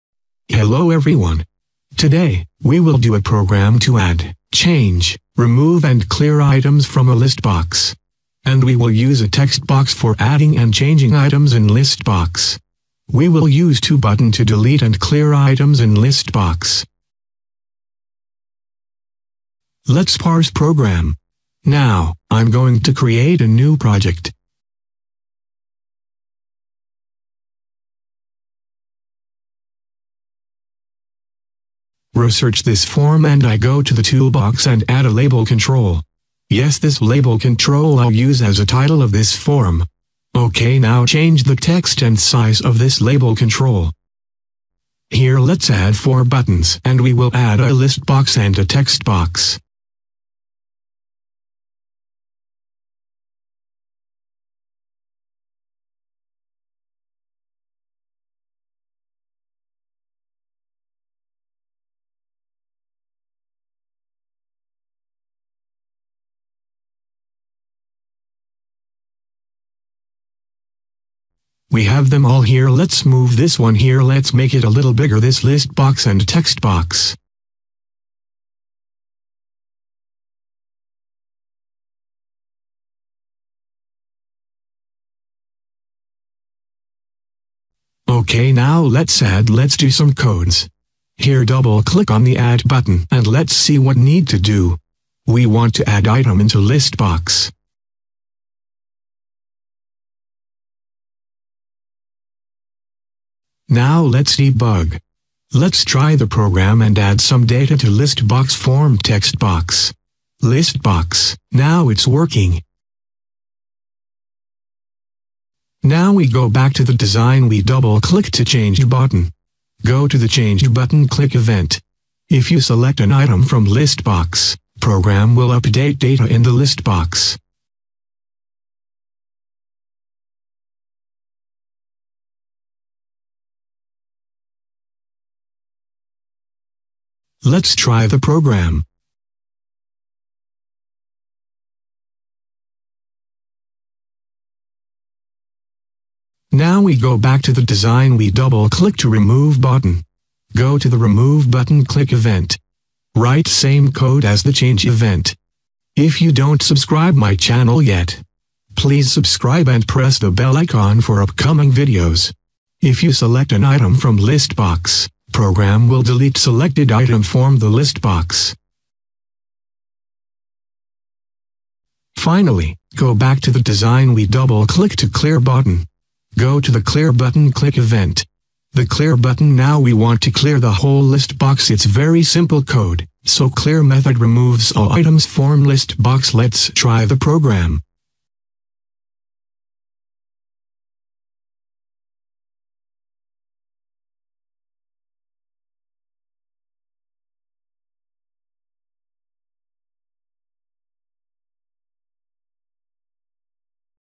C# Programming , Add, Change, Remove, and Clear Items in Listbox Using C#
Creating simple program to add, remove and clear items from a ListBox in C#.

Today, we will do a program to add, change, remove and clear items from a list box.

We will use a text box for adding and changing items in list box. We will use two button to delete and clear items in list box.